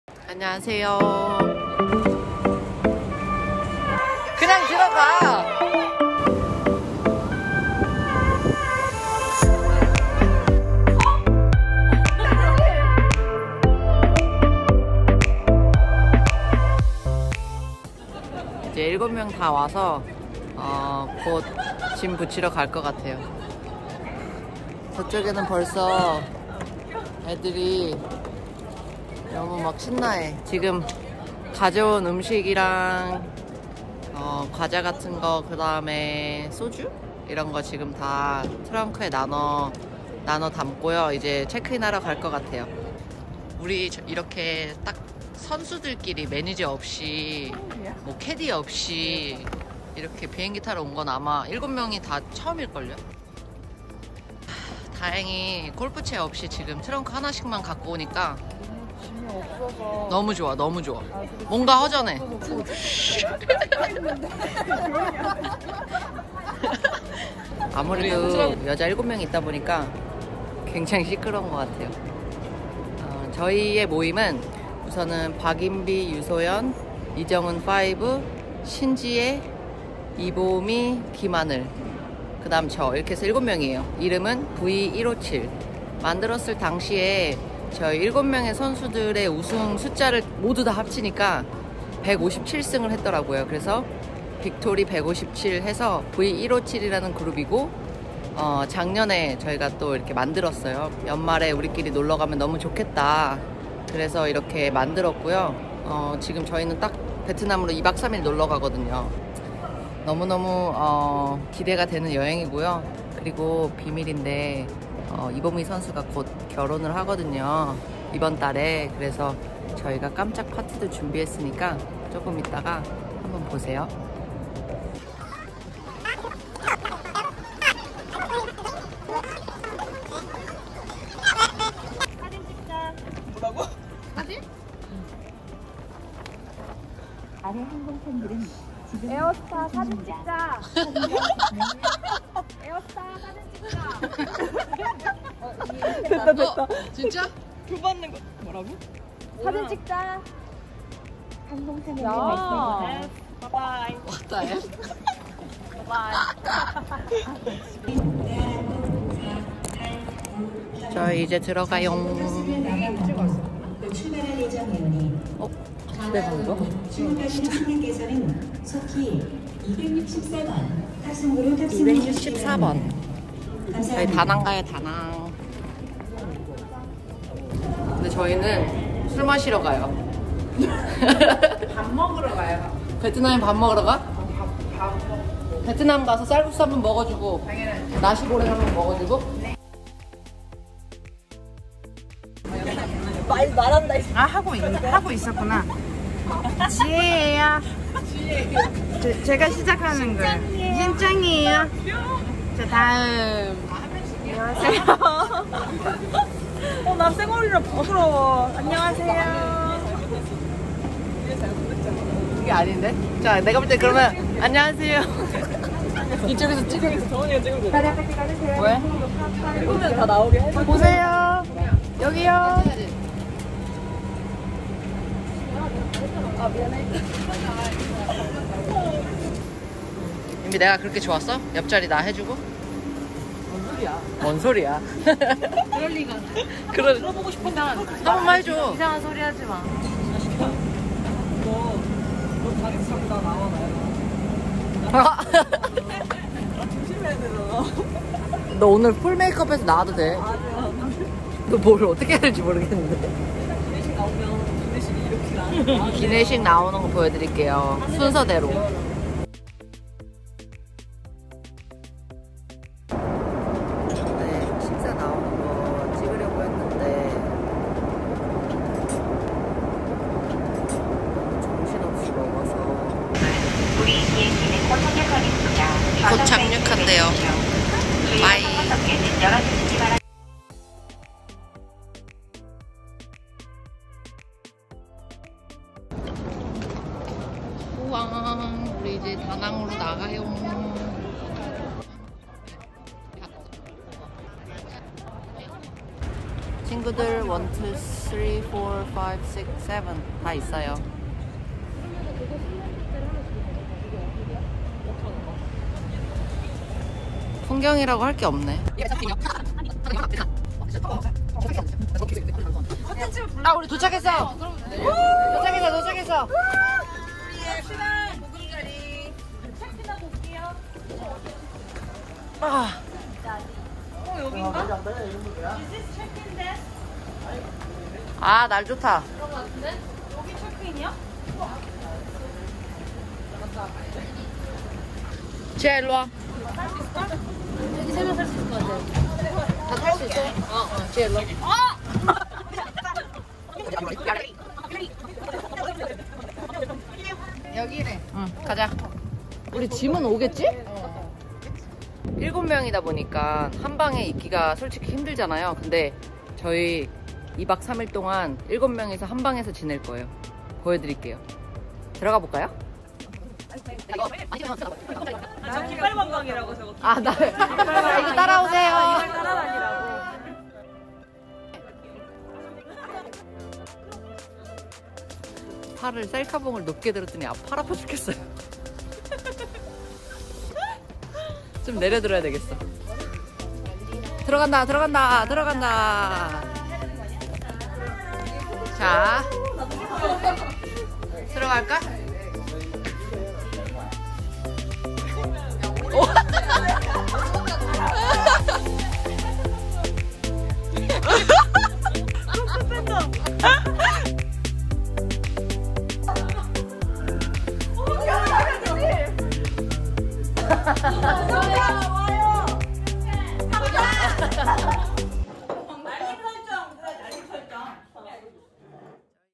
Vlog #02. (나름) 미녀 골프선수 7명이 모이면 무슨일이?? Trip to Vietnam with best friends!! 7 pro golfer!
안녕하세요
LPGA 선수 최나연 입니다.

제 일상을 담은 브이로그 영상입니다.
두번째 브이로그 영상으로 V157 맴버들과 함께한 베트남편을 소개하려고 합니다.
이보미 선수의 결혼 축하 파티(브라이덜 샤워)도 나오니 기대해주세요!

앞으로 더욱 재미있는 영상으로 찾아 오겠습니다!

▶Tag.